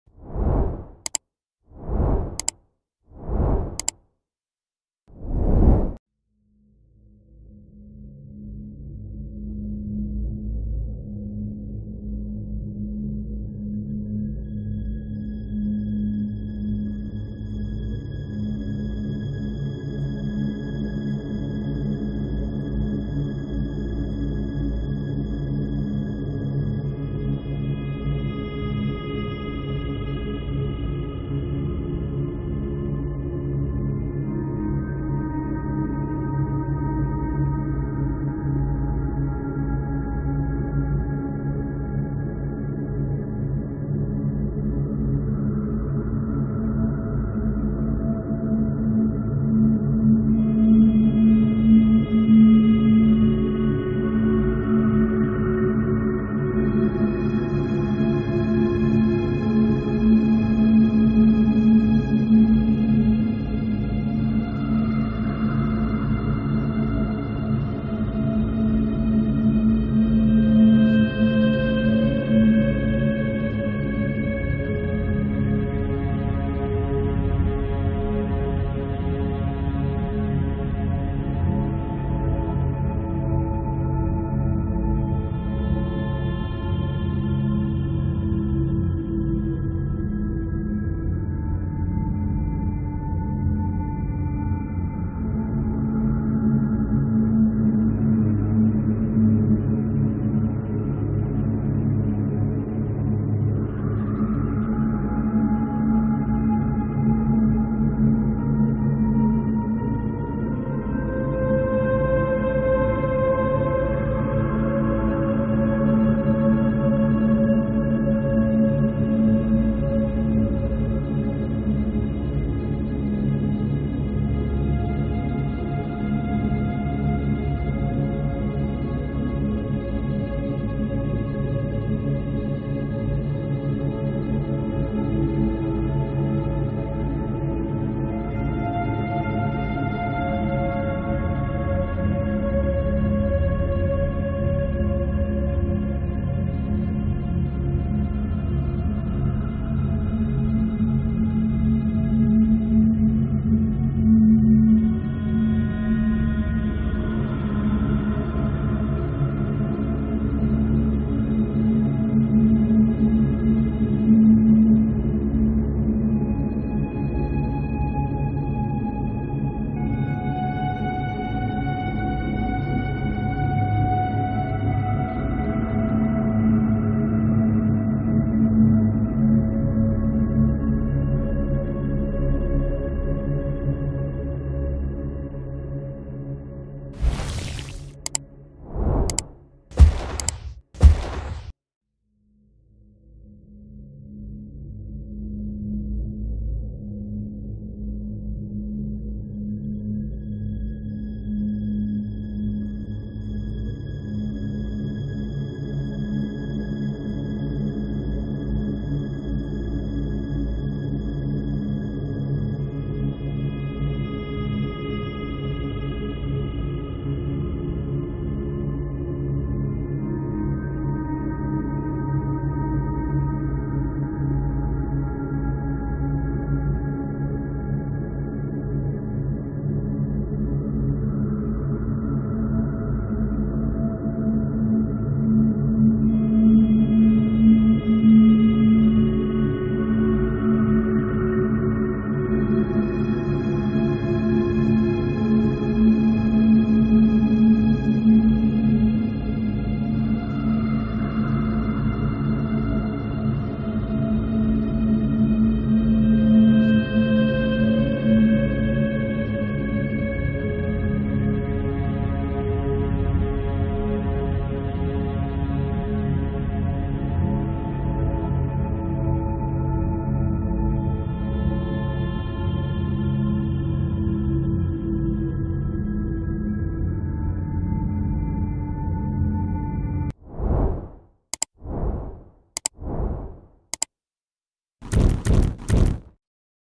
Washington DC the capital of united states
Washington, D.C., D.C. in full District of Columbia, city and capital of the United States of America. It is coextensive with the District of Columbia (the city is often referred to as simply D.C.) and is located on the northern shore of the Potomac River at the river’s navigation head—that is, the transshipment point between waterway and land transport. The state of Maryland borders the District of Columbia to the north, east, and west, and the state of Virginia borders the District on the southern shore of the Potomac River.
Please visit his Channel and leave a sub!